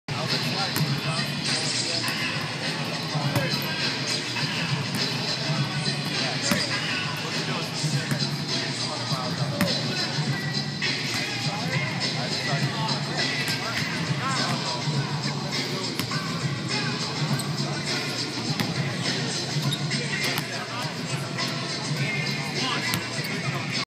SB Nation Boston: Ray Allen Warms Up Before Celtics Face He
Ray Allen shoots during warmups at TD Garden. Isn't it pretty?